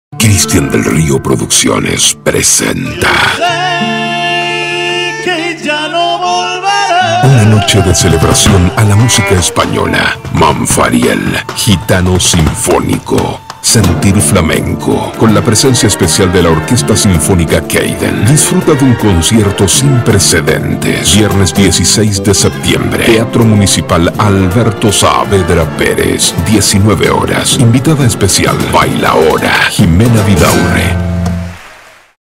Por primera vez en Bolivia, Cristian Del Rio presenta: MANFARIEL GITANO SINFÓNICO "SENTIR FLAMENCO"
🛑 Por primera vez en Bolivia, Cristian Del Rio presenta: MANFARIEL GITANO SINFÓNICO "SENTIR FLAMENCO". Única presentación VIERNES 16 de SEPTIEMBRE, Teatro Municipal "Alberto Saavedra Pérez" 19:00 Hrs.- Mayores Informes

Manfariel Gitano:
- Cristian Del Rio (Dirección, cante y guitarra)
- Luis Garzón "El Moro" (Cajón flamenco, armónica y coros)
- Oscar Chungara (1ra Flauta traversa)
- Luis Alarcón (2da Flauta traversa)
- Rodrigo Rojas (Bajo)
- Kantuta Cavour (Palmas y baile)

Bailaora Invitada: Ximena Vidaurre
Fagot: Joselyn Alarcón

Orquesta "Keyden" : 1ros violines, 2dos violines, violas, violonchelos, contrabajo, 1er Oboe, 2do Oboe